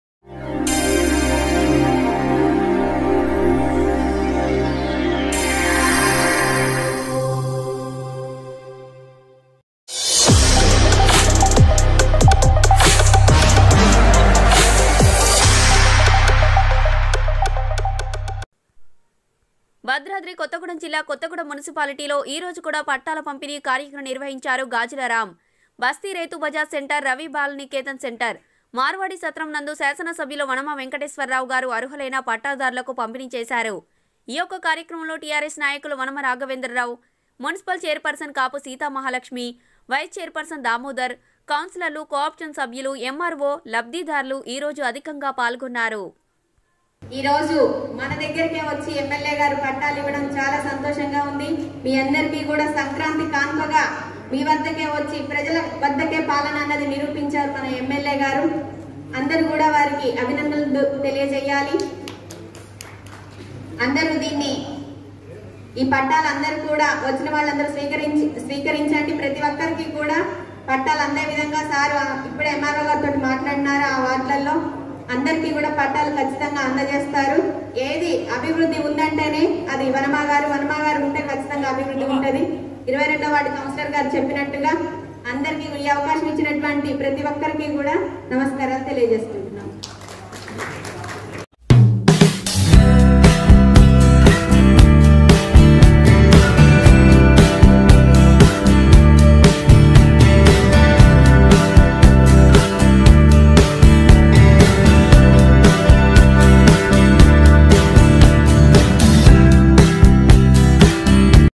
BADRADHRI || Kothagudem Muncipality lo Kuda Pattala Pampini Karikramam || 13//01//21
భద్రాద్రి కొత్తగూడెం జిల్లా కొత్తగూడెం మున్సిపాలిటీ లో ఈరోజు కూడా పట్టాల పంపిణీ కార్యక్రమం నిర్వహించారు గాజుల రామ్ బస్తి రైతు బజార్ సెంటర్.రవి బాల్ నికేతన్ సెంటర్.మార్వాడి సత్రం నందు శాసనసభ్యులు వనమా వెంకటేశ్వరరావు గారు అర్హులైన పట్టాదారు లకు పంపిణీ చేశారు ఈ యొక్క కార్యక్రమం లో టిఆర్ఎస్ నాయకులు వనమా రాఘవేంద్ర రావు గారు మున్సిపల్ చైర్ పర్సన్ కాపు సీతామాలక్ష్మి గారు వైస్ చైర్ పర్సన్ దామోదర్ గారు కౌన్సిలర్లు కో ఆప్షన్ సభ్యులు ఎమ్మార్వో గారు లబ్ధిదారులు ఈరోజు అధికంగా పాల్గొన్నారు